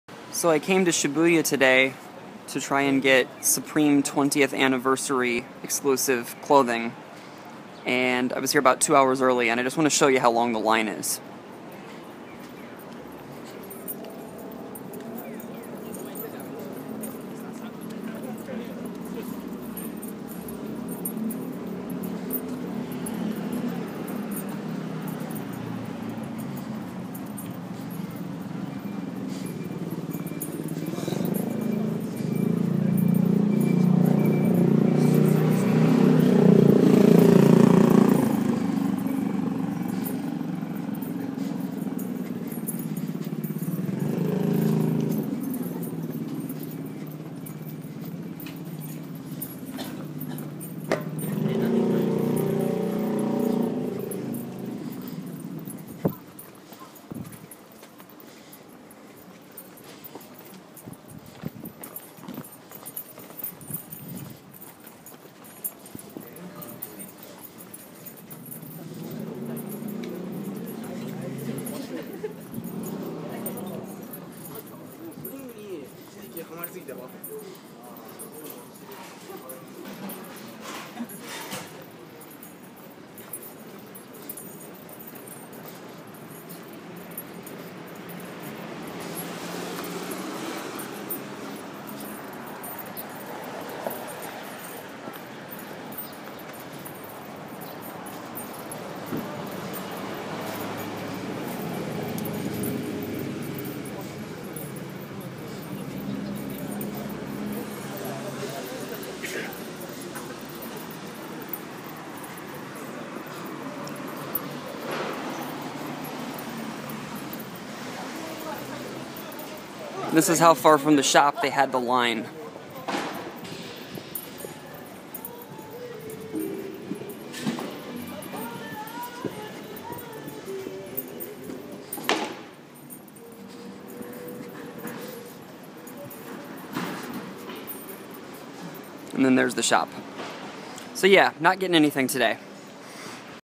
Supreme 20th Anniversary Line
People were lined up at 6 AM just to buy T-shirts...